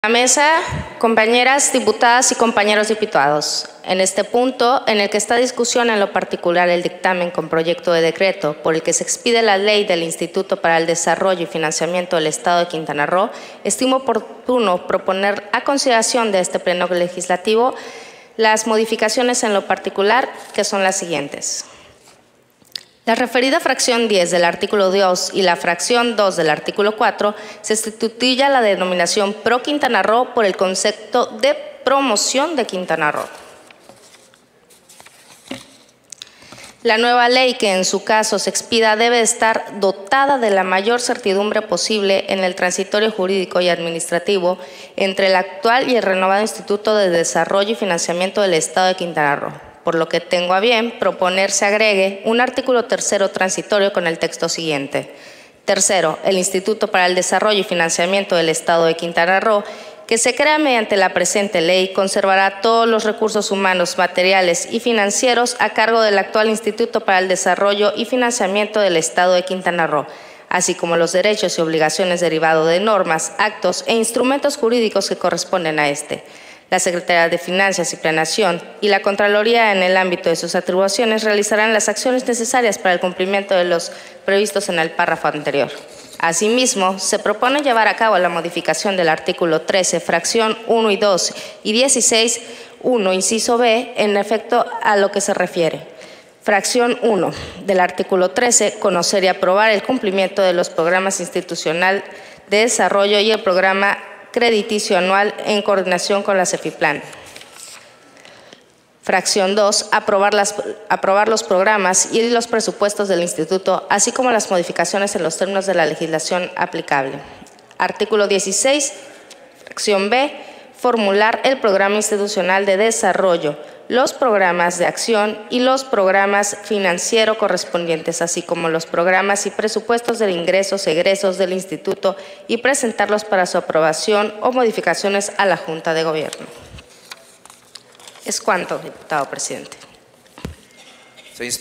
Intervención Dip. María José Osorio Rosas. PVEM (22/Diciembre/2023)
Intervención de la Dip. María José Osorio Rosas, del PVEM, en relación al Dictamen con Proyecto de Decreto por el que se expide la Ley del Instituto para el Desarrollo y Financiamiento del Estado de Quintana Roo. Publicado en la Gaceta Parlamentaria Año II número 13 Extraordinario del 20 diciembre de 2023.